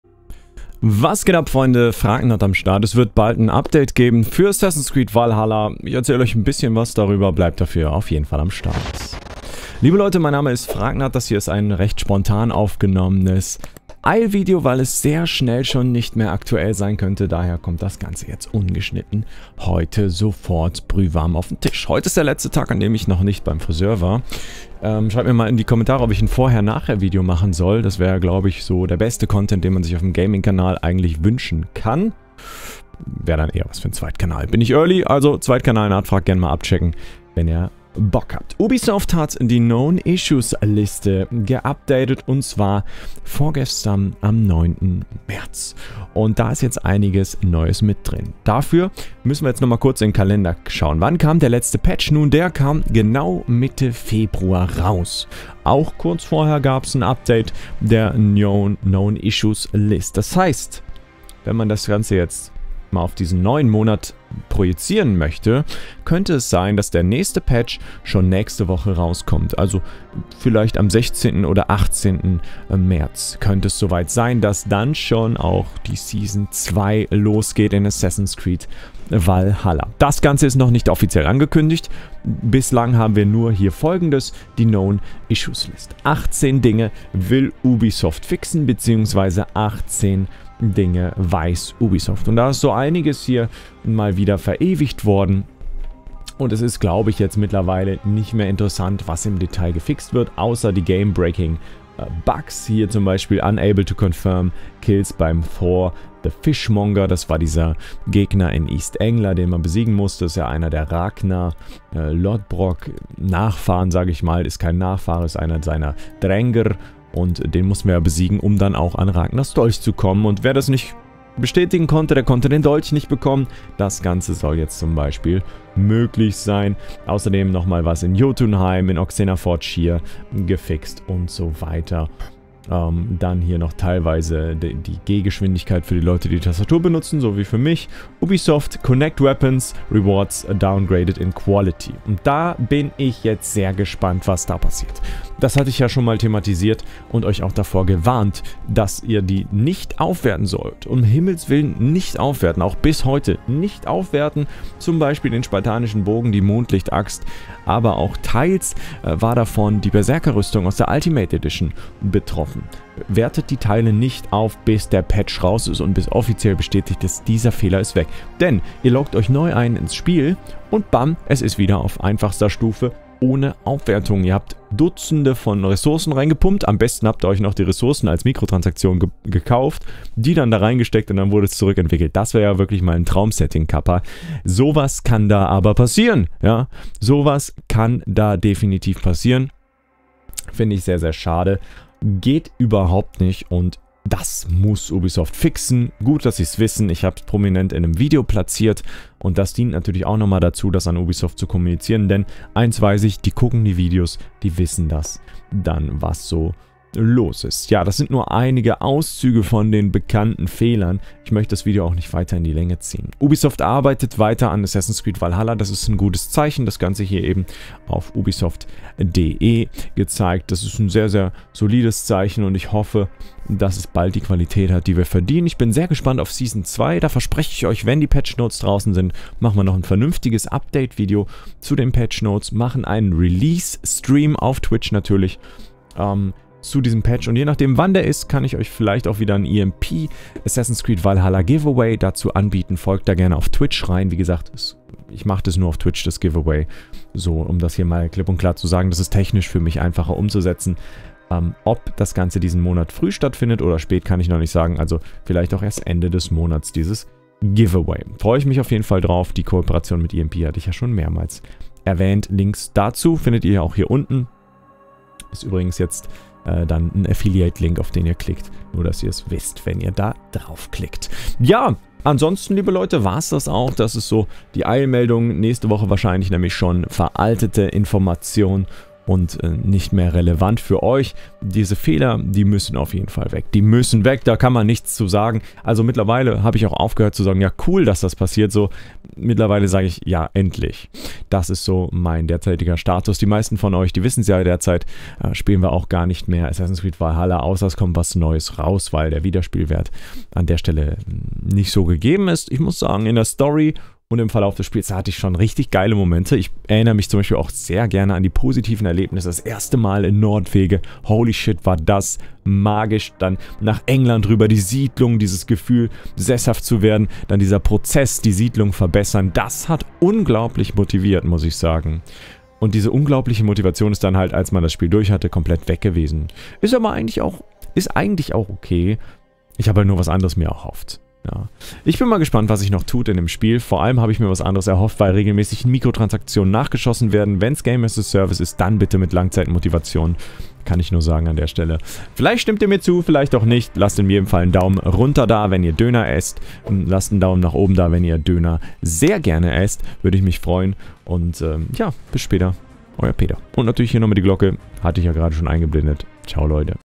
Neues Update für Assassin's Creed Valhalla in der Pipeline - Patch 1.2.0 kommt bald - Known Issues
Danke euch!